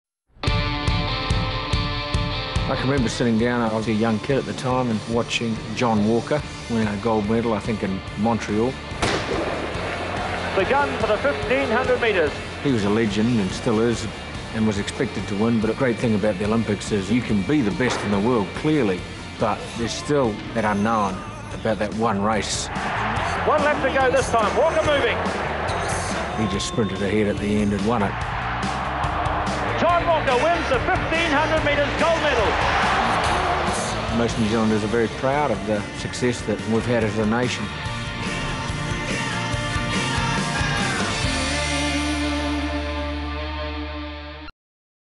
NZ2012.com Sir Russell Coutts on Sir John Walker
Olympian Sir Russell Coutts reflects on what makes him most proud to be a New Zealander.

New Zealanders are being asked to share their proudest moments with the New Zealand Olympic Team and inspire our athletes as they head to the London Olympic Games.